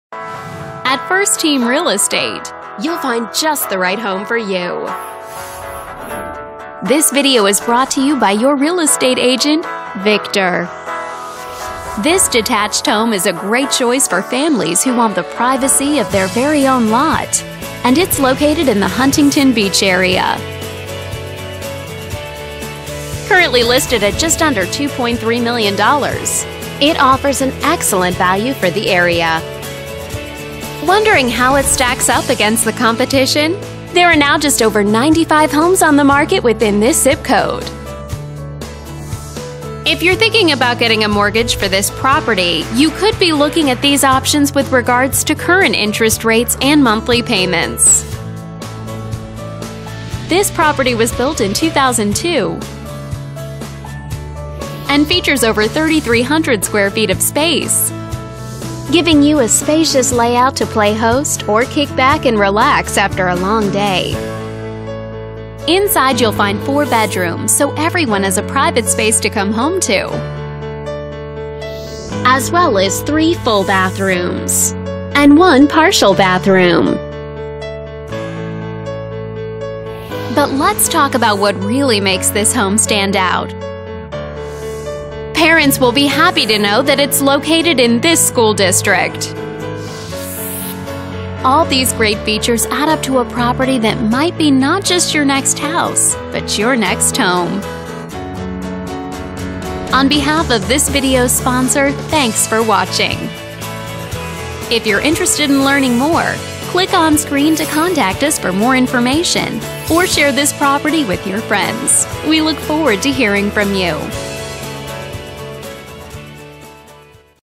17031 Marina Bay Drive
This luxurious waterfront home was reconstructed in 2002 and rests on of the largest cul-de-sac lots located in the 24hr gated community of Coral Cay that includes private tennis court and an exclusive beach. A private dock can accommodate a 70'boat. A separate and secured saltwater pool entertaining area with solar heating, spa, and fire pit. Enjoy the tranquil tropical front courtyard with Japanese bridge Koi pond and garden. A two story-high foyer greets you and opens into a spacious living room with Egyptian limestone fireplace. The formal dining room has a large window to capture the waterfall and waterfront view. The magnificent kitchen with top of the line appliances, granite counter top, expansive cooking island, double pantry, wine refrigerator & built-in refrigerator. Beautiful French limestone throughout downstairs with radiant heat. Separate & private master suite with balcony overlooking the bay, luxurious travertine bath, shower & spa tub, radiant heat, his & her vanity.